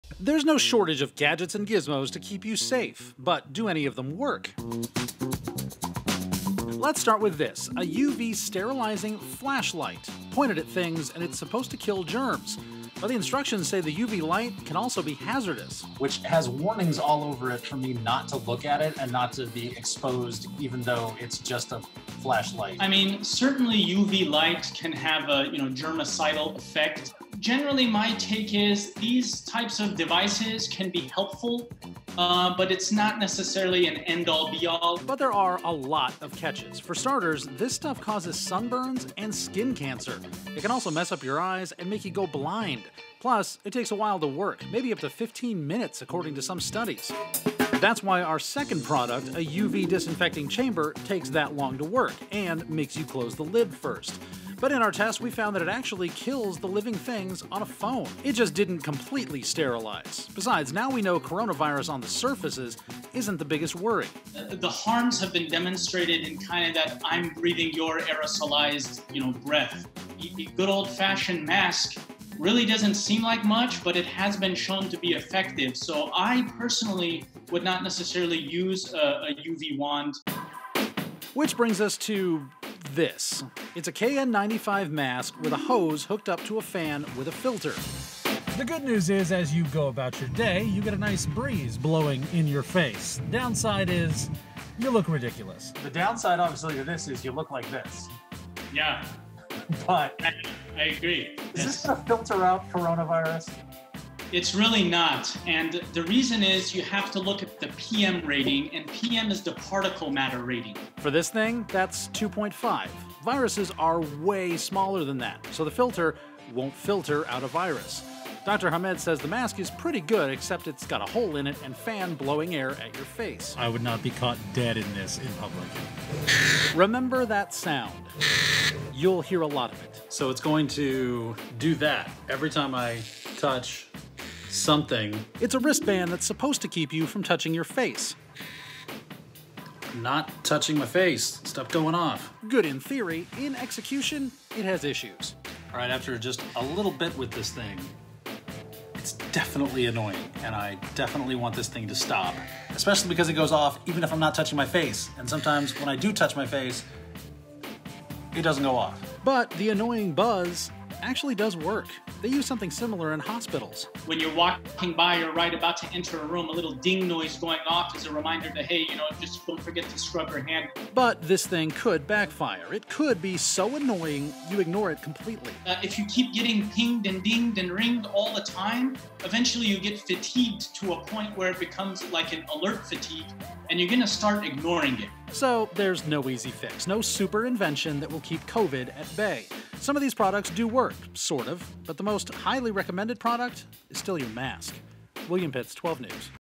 3 germ-killing gadgets tested to see how effective they really are
We all know about face masks and hand sanitizer, tut there are a ton of products flooding the market right now.

They claim to offer you more protection, and you can still find some of them before seeing your family.

Team 12's William Pitts is putting three germ gadgets to the test.